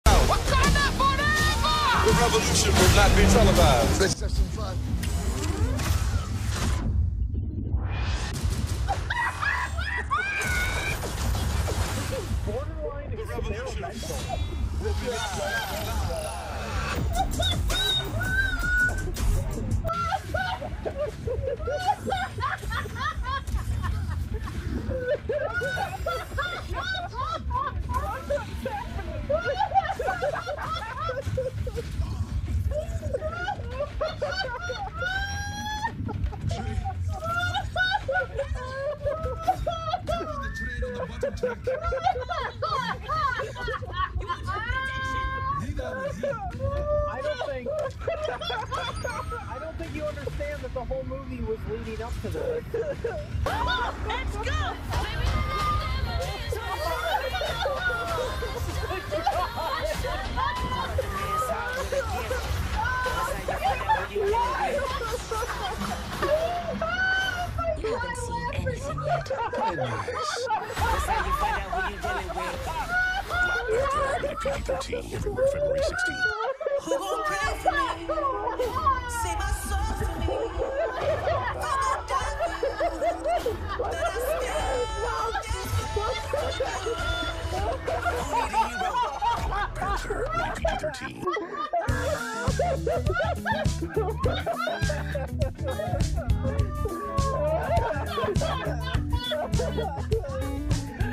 RedLetterMedia Reacts to Black Panther's CGI
Me love me some bad CG.